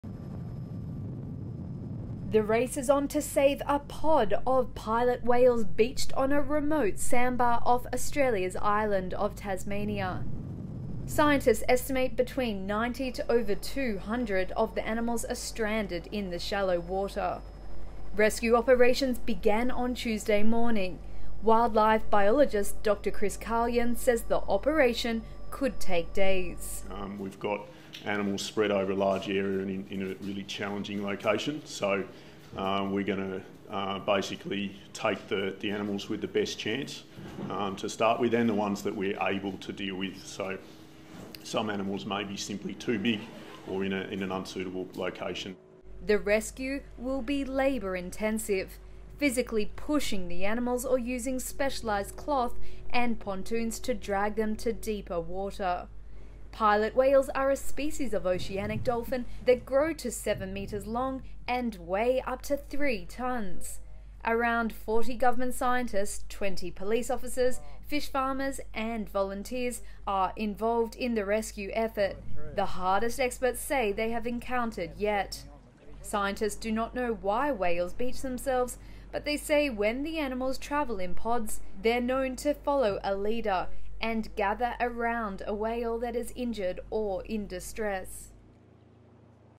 Rescuers try to save Australia's beached whales
Rescue operations begun on Tuesday morning to save over 200 whales stranded on a sandbar off the remote west coast of the Australian island of Tasmania.